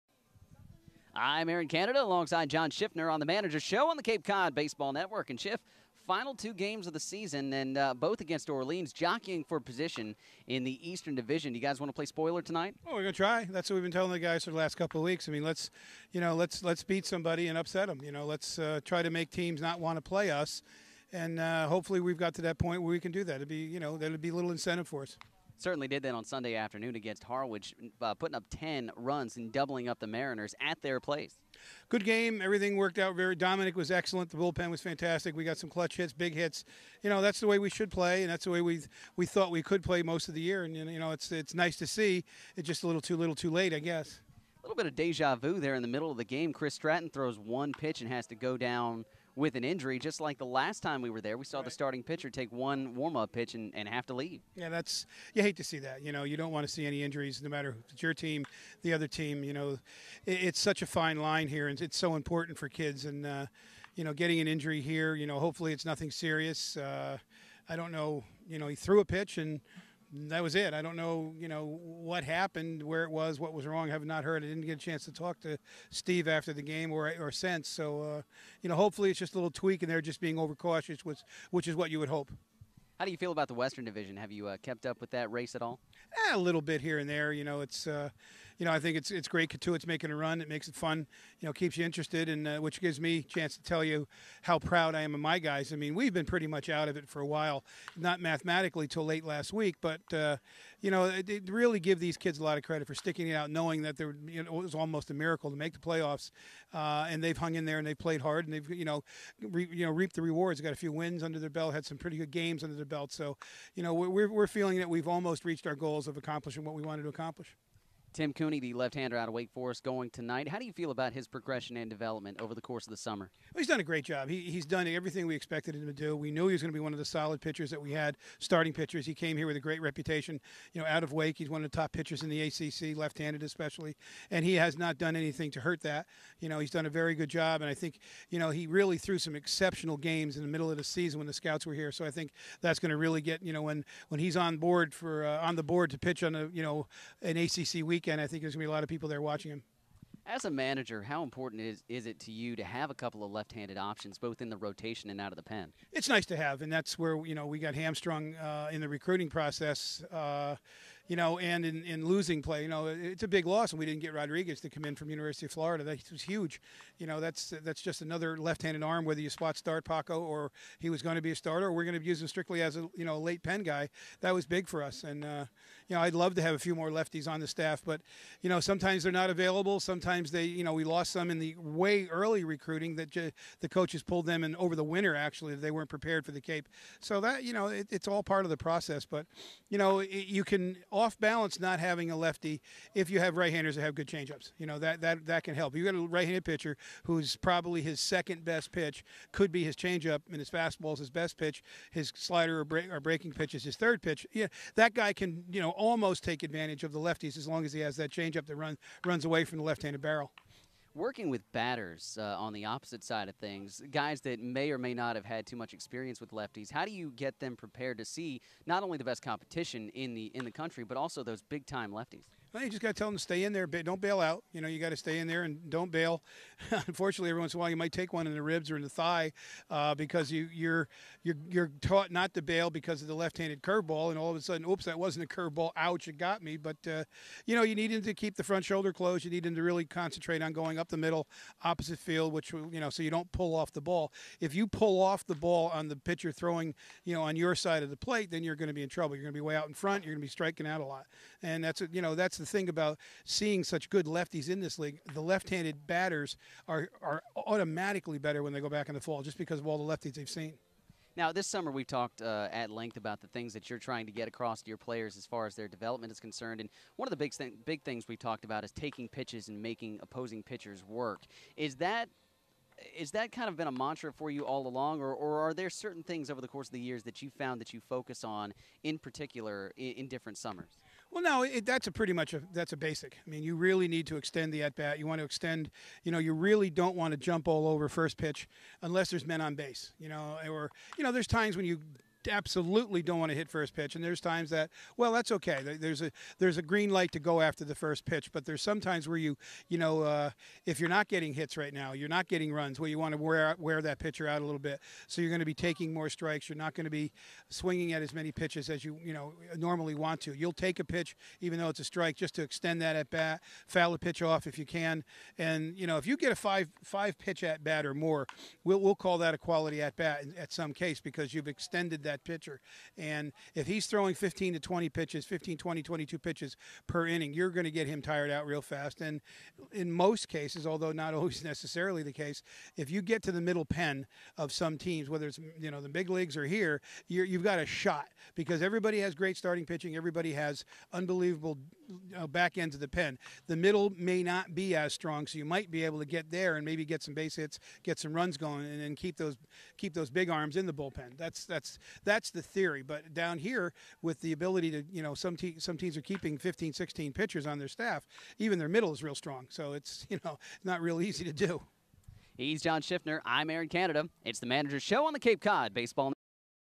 Game 43 Manager's Show
Aaron Canada talks to Anglers manager John Schiffner prior to August 2nd's rainout vs. Orleans.